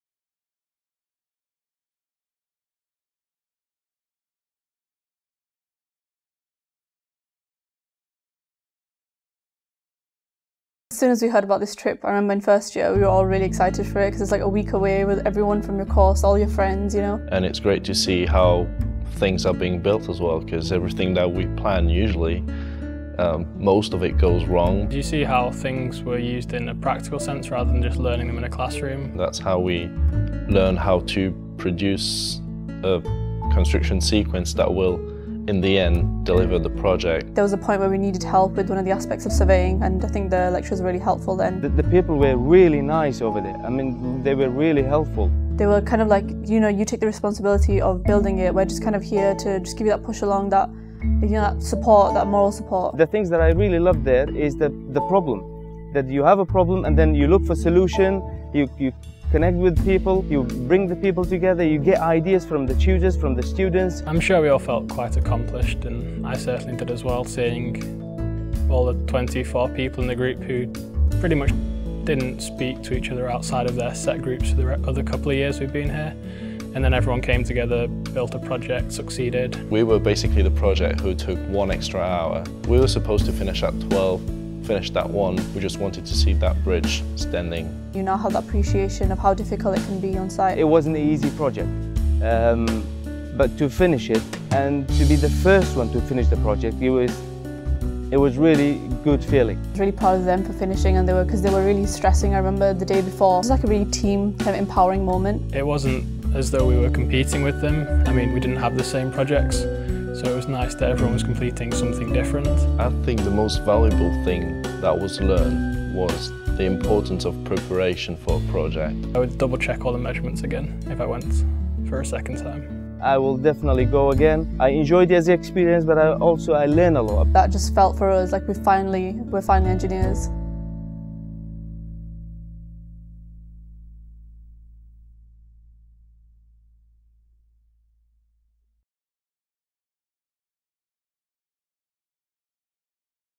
Civil Engineering - Constructionarium 2016
Second year students Arooj Shah, Rémi Sauffisseau, Hemen Mohammed and Edward Cook talk about their experiences preparing for and working on the civil engineering projects at the Constructionarium.